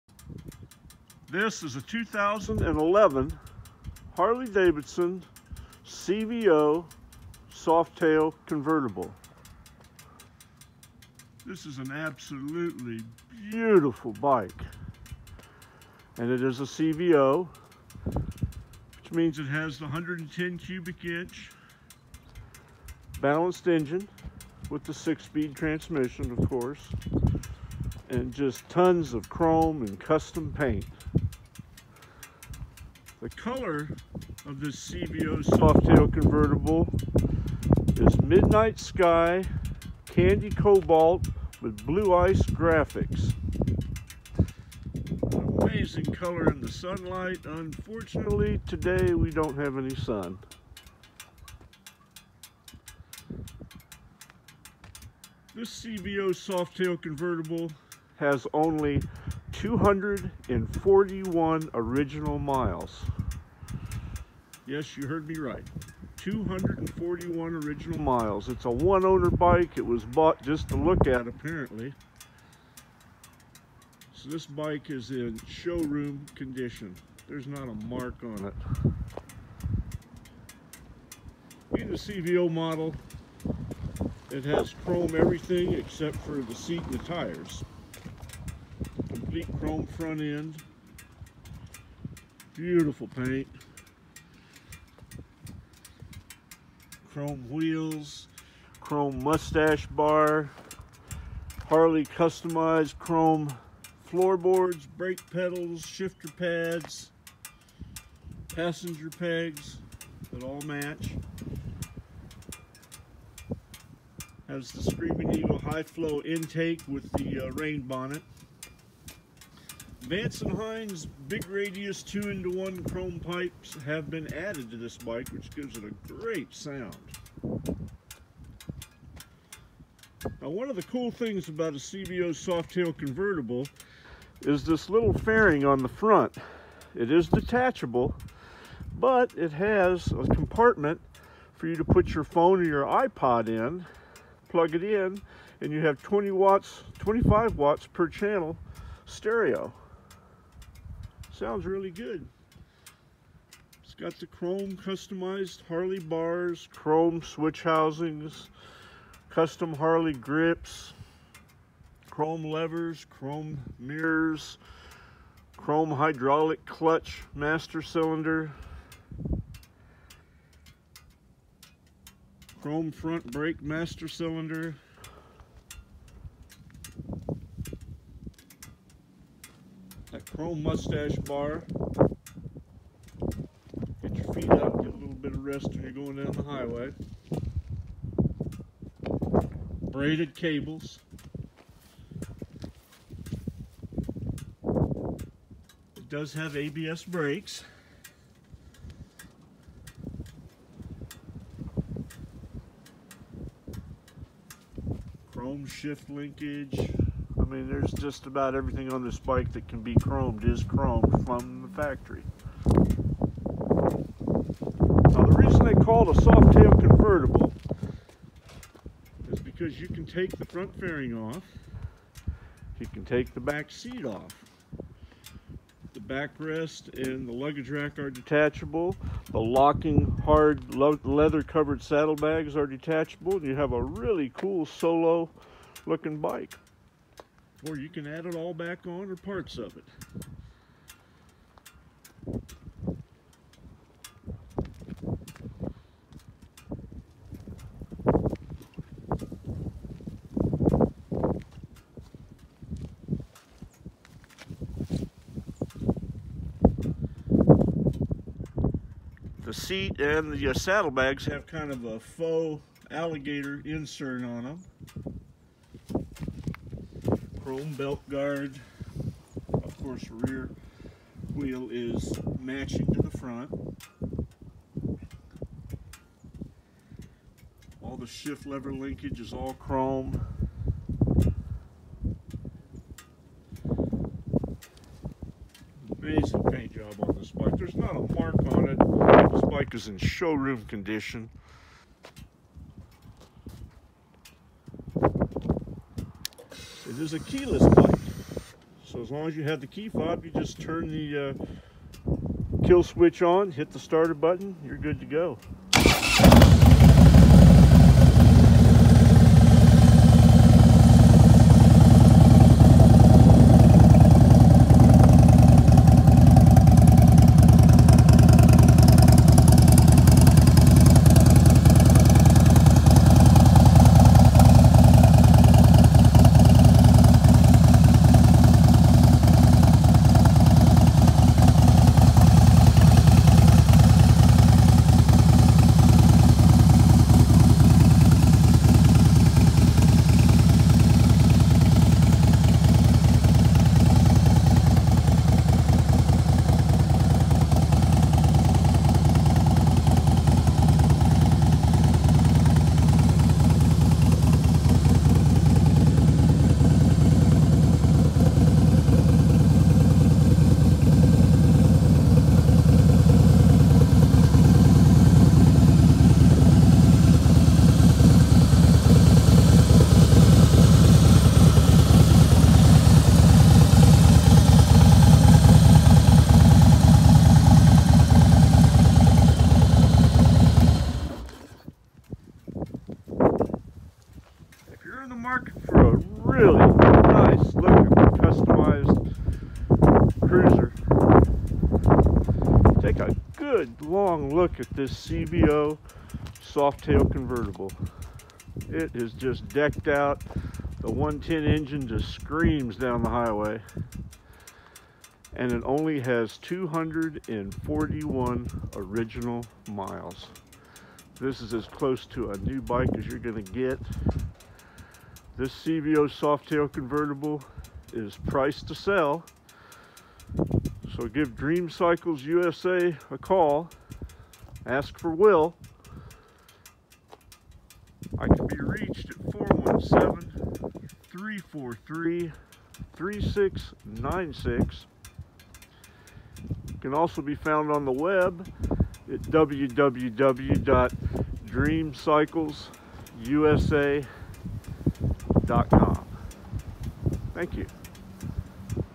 2011 CVO Softail Convertible
2011 Harley Davidson CVO Softail Convertible  FLSTSE  -  SOLD

This Screamin Eagle Softail Convertible is a very LIMITED EDITION model with only 547 built with the Midnight Sky / Candy Cobalt with Blue Ice graphics paint scheme.
With only “241 ORIGINAL MILES” on the odometer this beauty is begging to be to ridden. The 110” Sreamin Eagle engine and 6 speed transmission will power you down the road and keep you at the front of pack!

As with all the CVO models that Harley Davidson produces this bike is loaded with chrome options. Here is a list of the many Chrome options included on this bike.
 Chrome Accessories
- Clutch line and brake line clamps
- Derby and timer covers with engine turn inserts
- Headlight trim ring, nacelle and back plate
- Front sliders
- Wheel spacers
- Handlebar risers and caps
- Shifter, rear brake lever and forward control mounts
- Coil cover and voltage regulator
- Rear brake master cylinder cover
- Switch housings
- Upper belt guard and debris deflector
- Sprocket bolt covers
- Valve stem caps
- Front and rear axle nut covers
- Pivot shaft covers
o Black high-torque 1.4kW starter
o High-performance clutch with hydraulic actuation
o Harley-Davidson® Smart Security System with siren
Other highlights of this Cvo Softail Convertible are:
NEW Electronic throttle control with cruise control standard
NEW Anti-lock Braking System (ABS) standard
NEW Detachable compact fairing with integrated audio system featuring 3.5-inch 2-way speakers, 20W per channel amp with hookup for your Ipod or Phone .
NEW Alligator-style leather inserts on the detachable saddlebags, leather seat, passenger pillion and backrest pad
NEW Chrome, 1.25-inch welded mini-ape handlebar
NEW Keyless ignition system with CVO™ backlit medallion on the tank console
NEW Extended jiffy leg is easier to reach
o Detachable leather-covered saddlebags, passenger pillion, upright and backrest pad, and fairing
o Chrome aluminum 18-inch Stinger wheels with matching Stinger chrome sprocket and Stinger floating brake rotors
o 200/50R18 radial rear tire
o Rear light bar with integrated stop/turn/tail lights
o Combination digital speedometer with analog tachometer
o High-flow Ventilator engine air intake with alligator-style insert and rain sock
o Color-coordinated frame and swingarm, frame inserts, saddlebag brackets and horseshoe oil tank
o Screamin' Eagle® Twin Cam 110B™ Granite powder-coated powertrain with 110 Screamin' Eagle identifiers on cylinder heads
o Rumble Collection grips, floor boards, passenger and shifter pegs, brake pedal pad and mirrors
Vance & Hines Chrome Big Radius 2 into 1 Exhaust

For purchase information please contact Will at Dream Cycles USA at